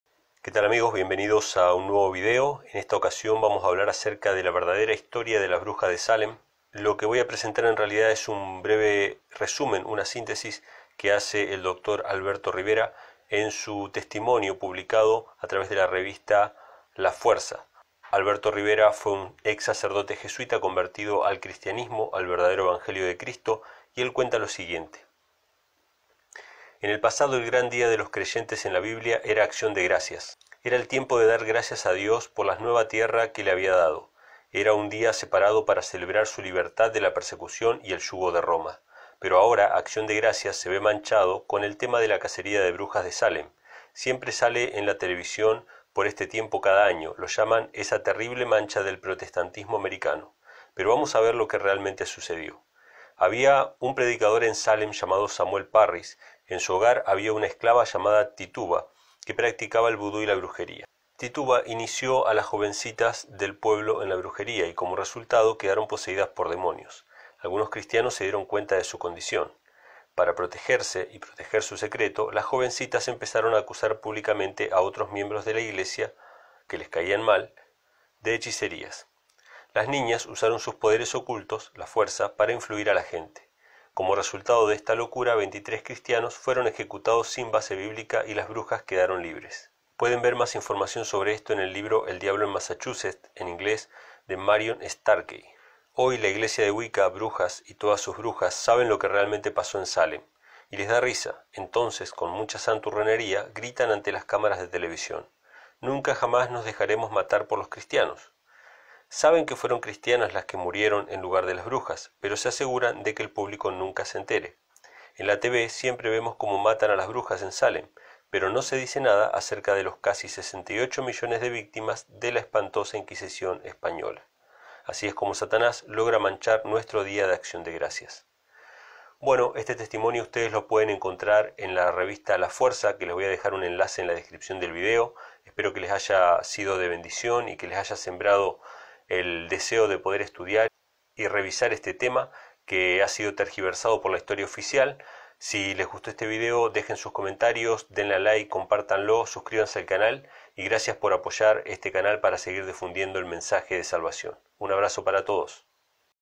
La Verdadera Historia de las Brujas de Salem
La historia que Hollywood no te cuenta. Comics de Alberto Rivera:
Fuente: The Devil in Massachusetts: A Modern Inquiry in the Salem Witch Trials de Marion L. Starkey

Colabora!!

Descarga mis libros:
El Único Hijo